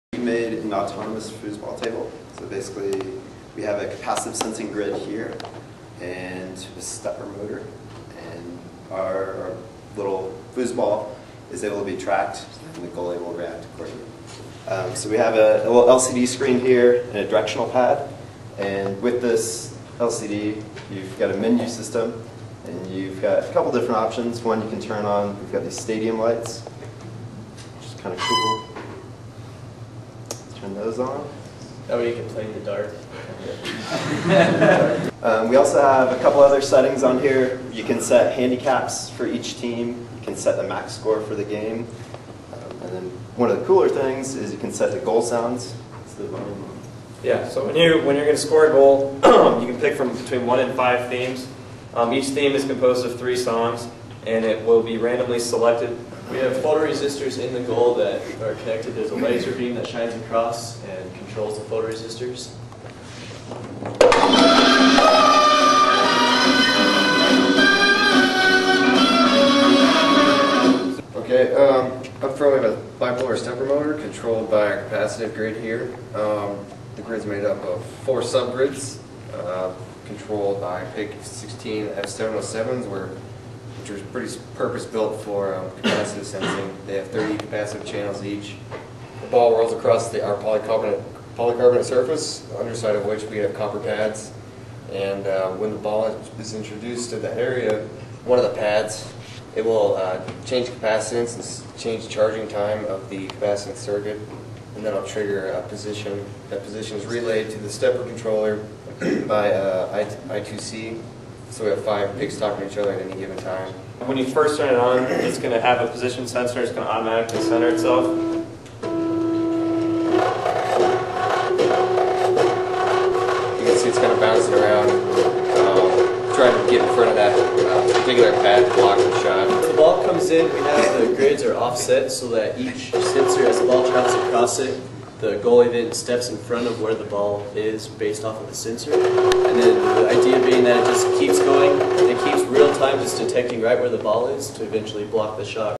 Foosball mechatronics project
Class project for Fall-2012 Colorado State University MECH307 Mechatronics course.